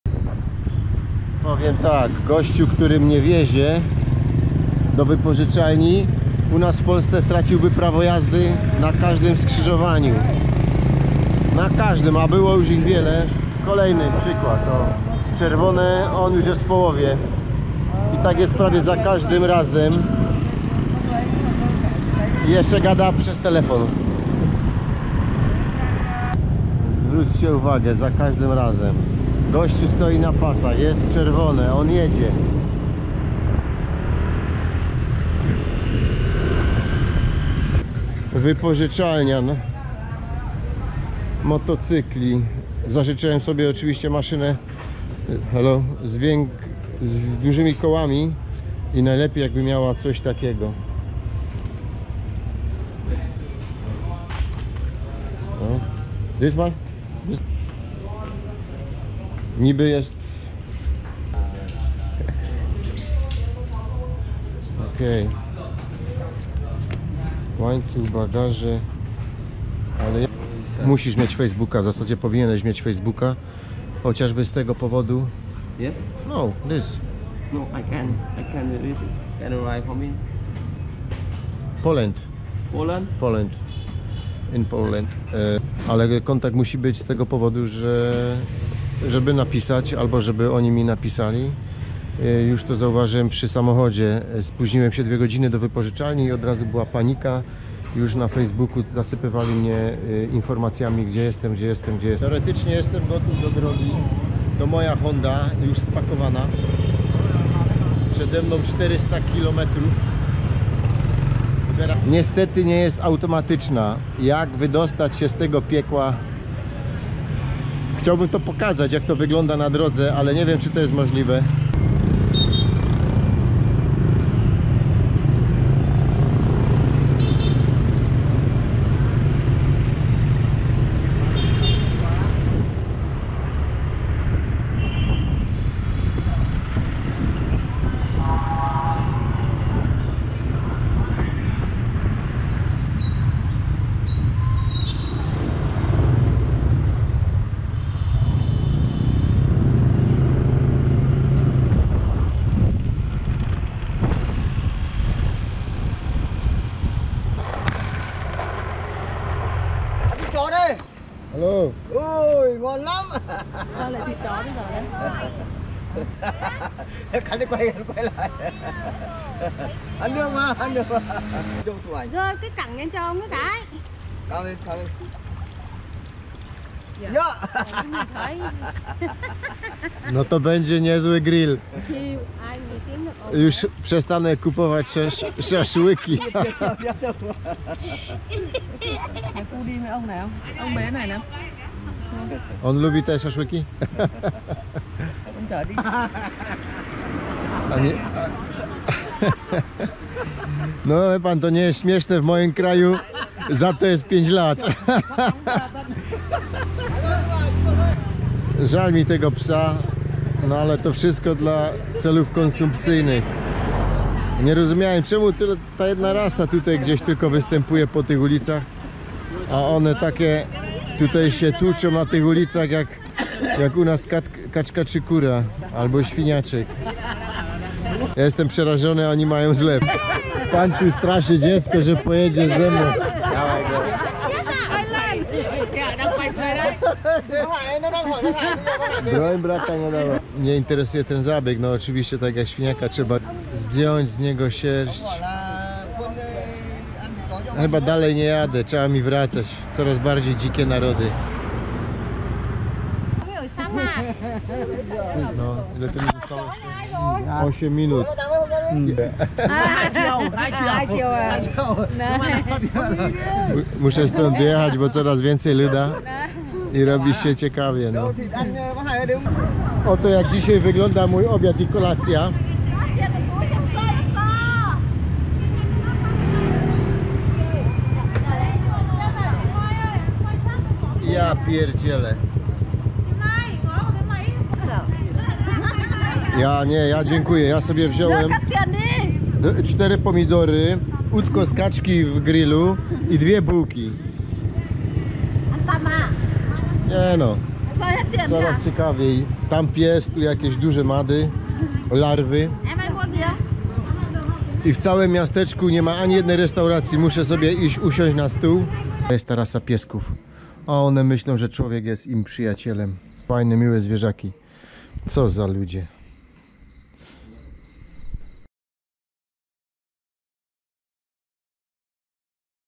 Wietnam -zjedli psa TVJP 18
Autor: Jan Płonka TVJP
Kupowanie bazarowych szaszłyków zawsze w Azji jest wątpliwe.